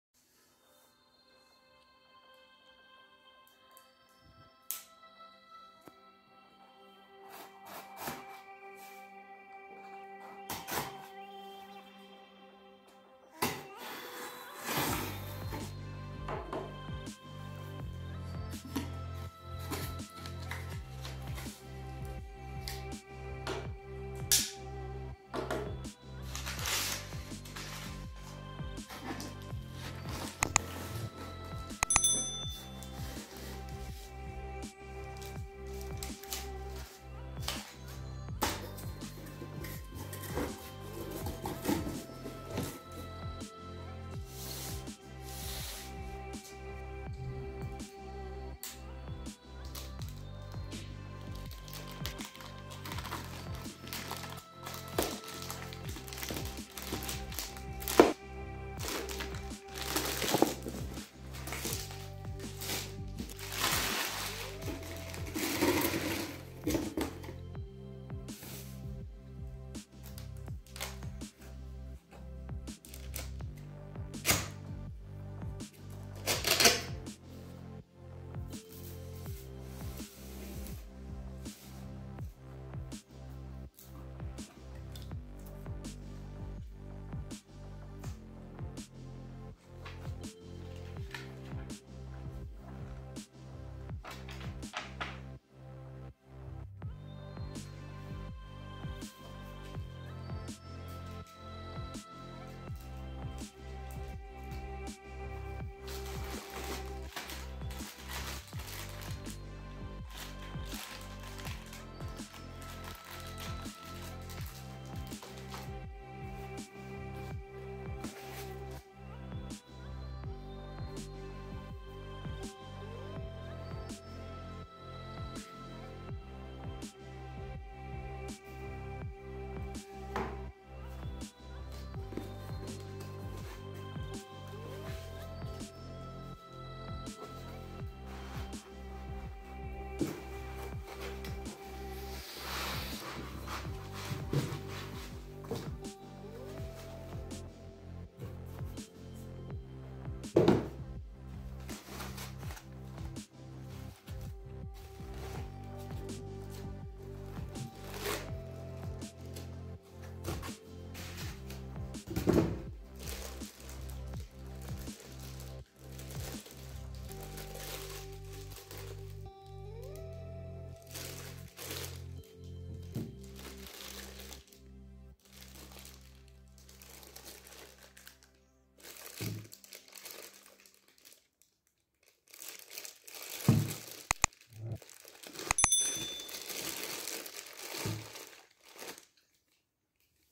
انباکسین رادیو بلوتوث موتر گلف ۶
سلام  یک رادیو موتر خریداری کردم از آمازون برای موتر گولف ۶